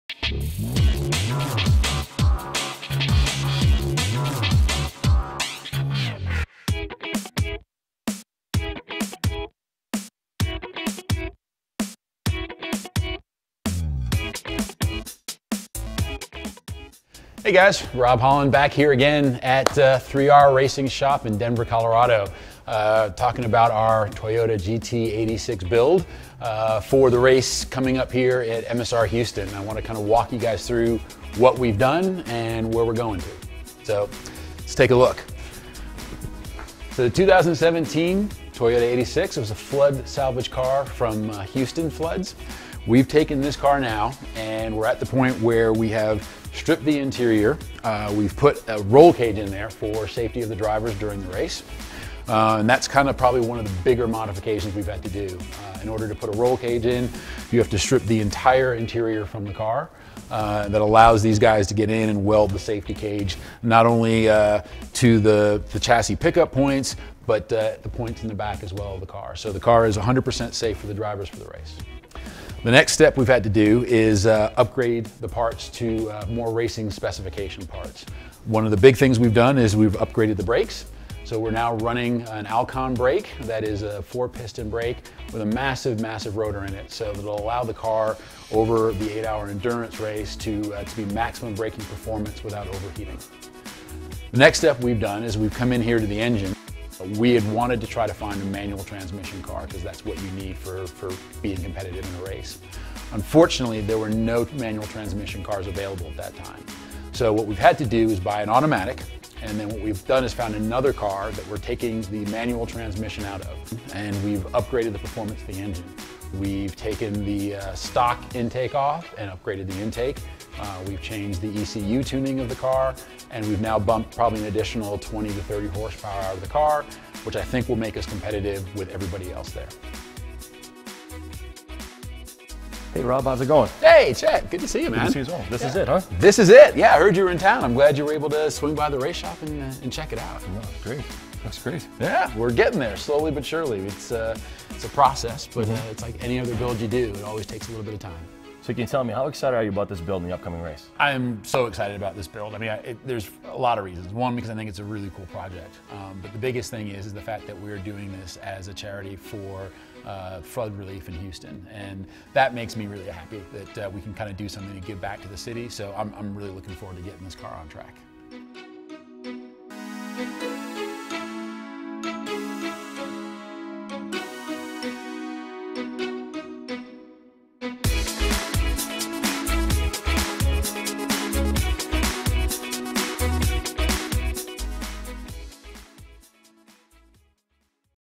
Episode 3 - Flood to Track | Transformation
Watch Robb's team take this car from head-turning, branded title, daily-driver to a race car, ready to compete at the WRL endurance race being held at MSR Houston.